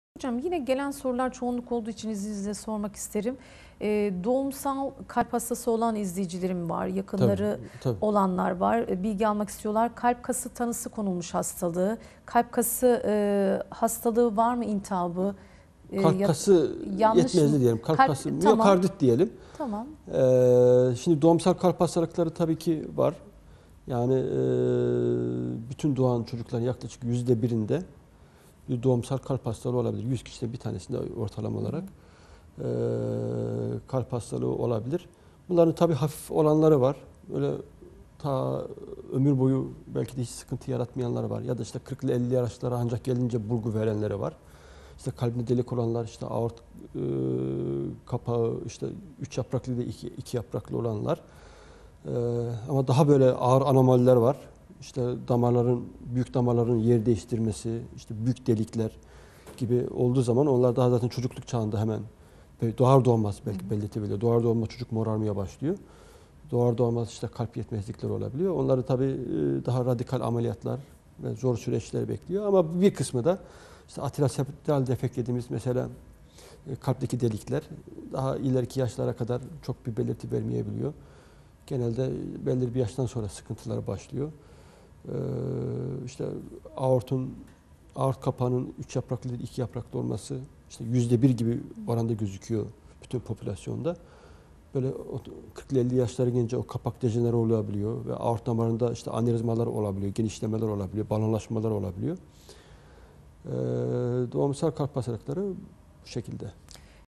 Kalp Ve Damar Hastalıkları Uzmanı Prof. Dr. Ali Rıza Cenal '' Kalp Kası '' anlatıyor
Avrasya Hastanesi Radyasyon Onkoloji Uzmanı Prof. Dr. Ali Rıza Cenal '' Kalp Kası '' hakkında detayı bilgiler veriyor